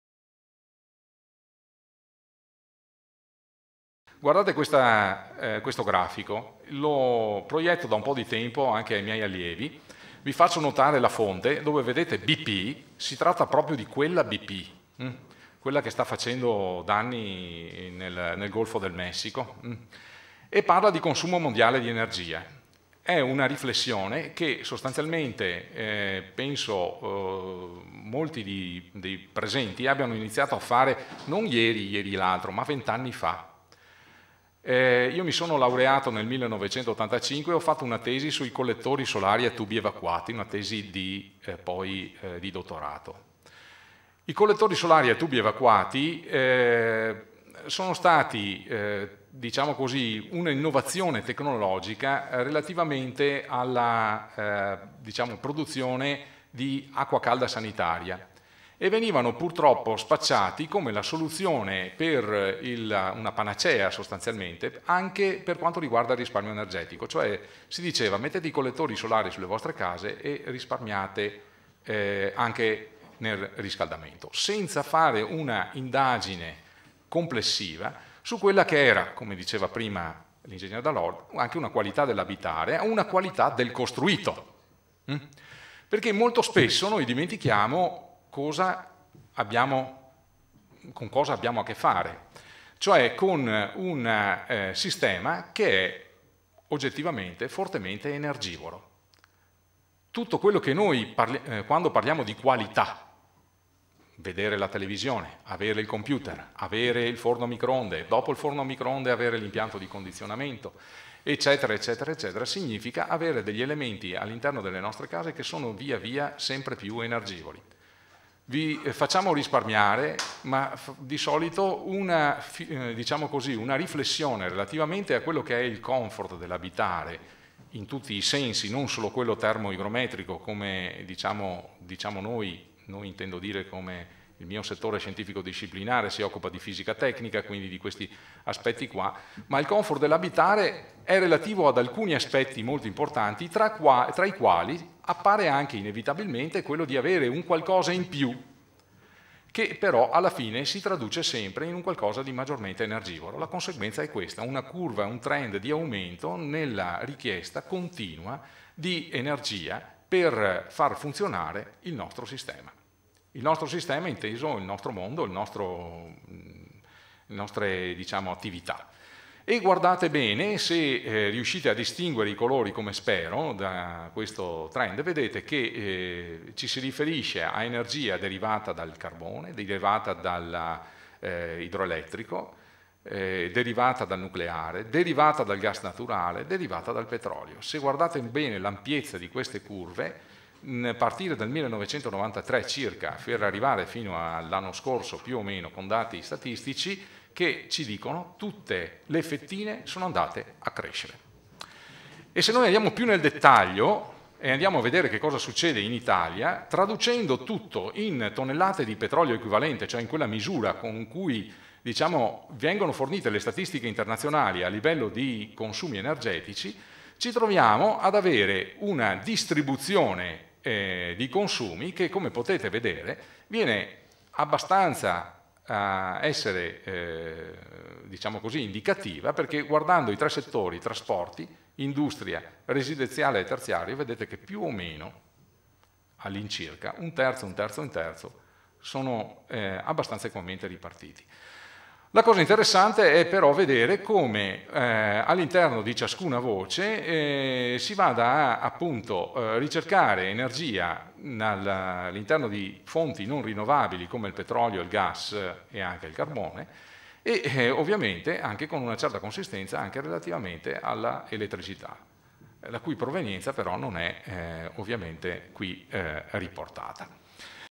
EDILIZIA SOSTENIBILE: INIZIATIVE LEGISLATIVE
Dott. Ing. Prof. Piercarlo Romagnoni (IUAV di Venezia - Dipartimento di Costruzione dell'Architettura)